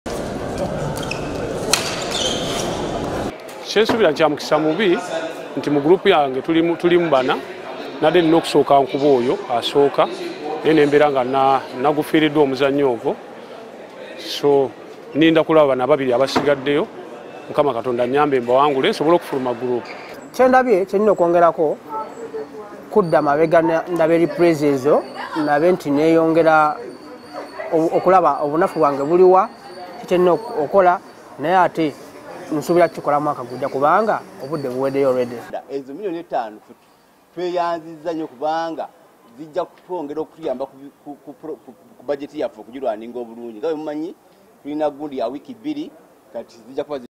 PARA BADMINTON: Empaka ziyingidde olunaku olw'okubiri
Empaka za Uganda Para Badminton International Championship, ziyingide olunaku olw’okubiri olwa leero e Lugogo, nga abazannyi abaava munsi 20 battunka. Wabula webuwungeeredde olwaleero nga abazannyi ba Uganda empaka tezibatambulira bulungi kubanga bangi kubo bawanguddwa. Ebyo nga biri bityo bo abategesi b'empaka zino bakubiddwa enkata ya bukadde butano okuva mu bank ya Baroda zibayambeko mu nteekateeka eno.